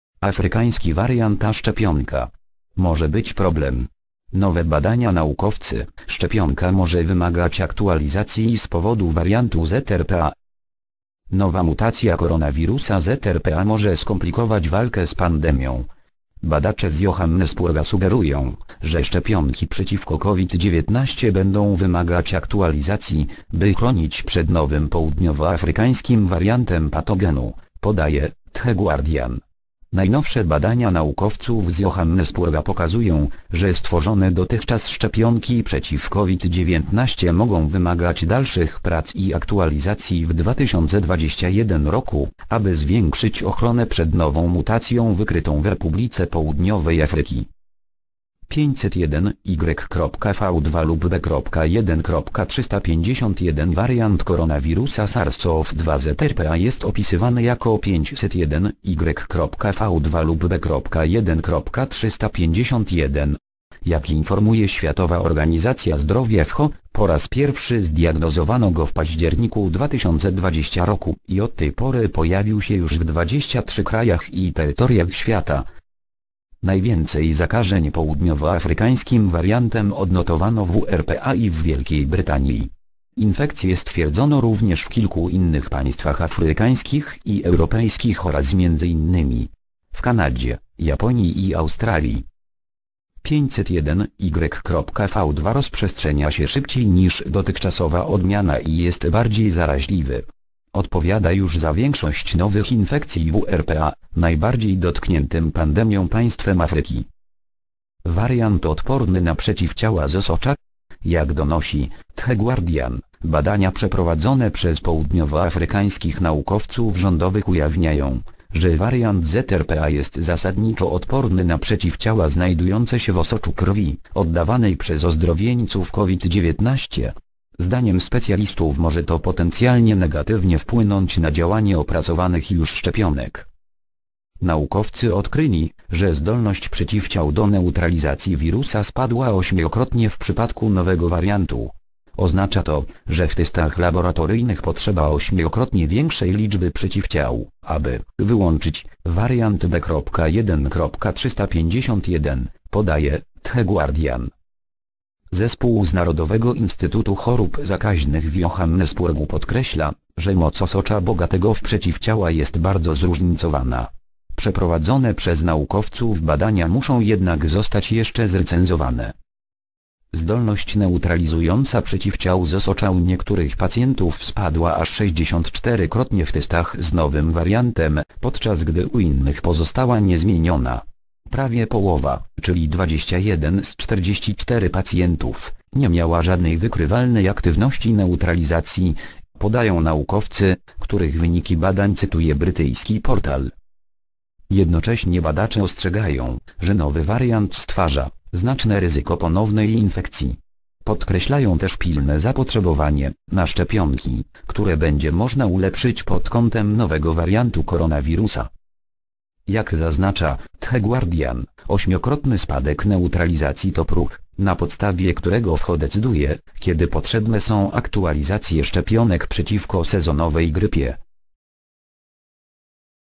Afrykański wariant a szczepionka. Może być problem. Nowe badania.
Naukowcy: Szczepionka może wymagać aktualizacji z powodu wariantu z RPA.

  (fot. Prapass Pulsub/Getty Images)